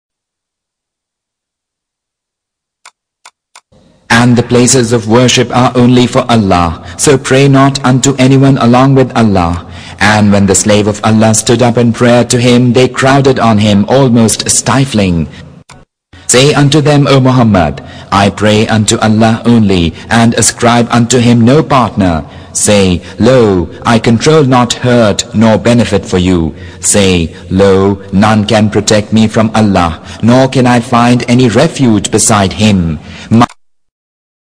Verses - the mosques are for Allah
the places of worship are only for Allah, so pray not unto anyone along with Allah.

Mosque
Church
Temple

Ground Zero welcome mosque because that is a religion not a life style
america has 8% of them.